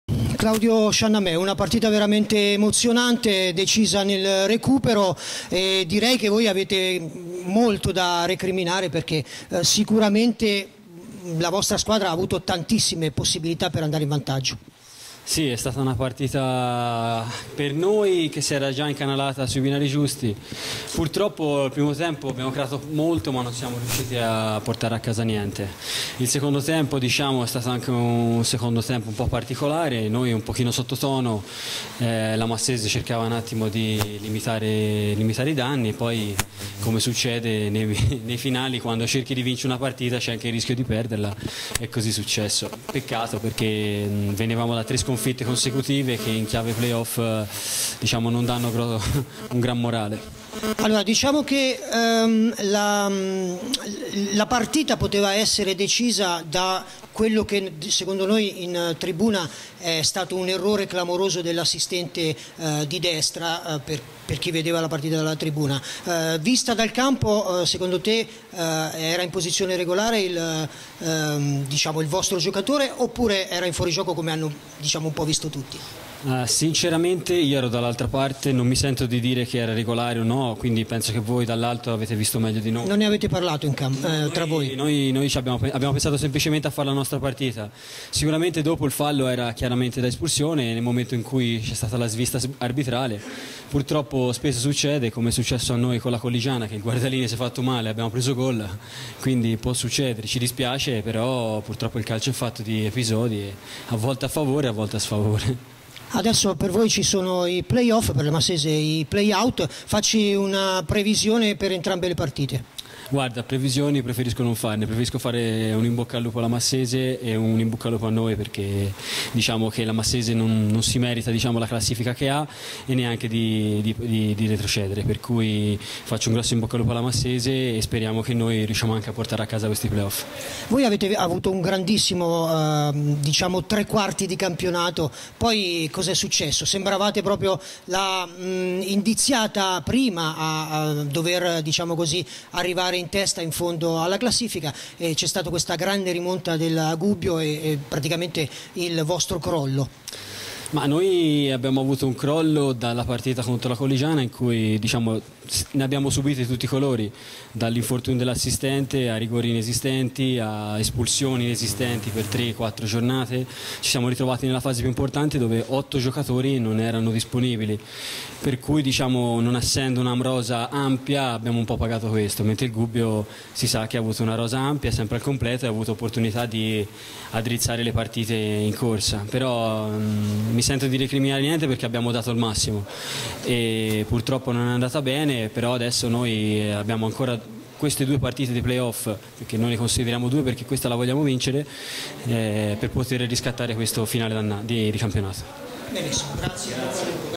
Massese Gavorrano 1 - 0 intervista a Claudio Sciannamè dello 08/05/16
Massese Gavorrano 1 - 0 interista a Claudio Sciannamè, difensore della Gavorrano, dello 08/05/16